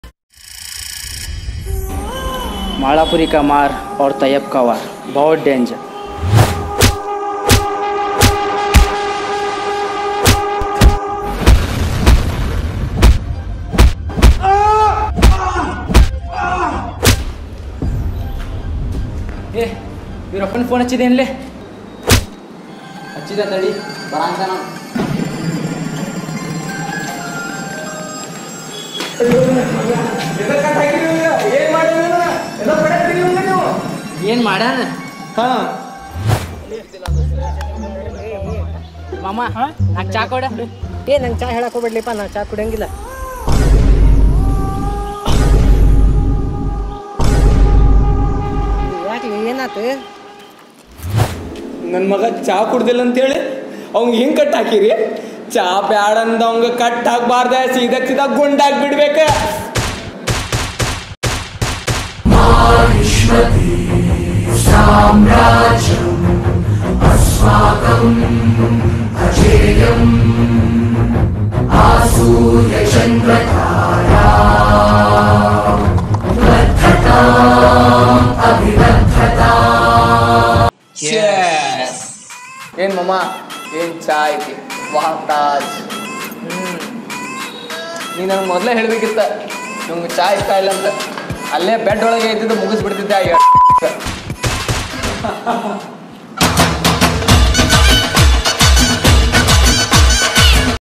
THE REVENGE | DreamerZ
Hey Guyz we are back with another episode called "THE REVENGE"
The video is about the madness of uttara karnataka people towards Tea (Chai).

Hope you enjoyed the video,v

(Note : This video is dubbed in kannada from ashish chanchlani's video "Revenge".)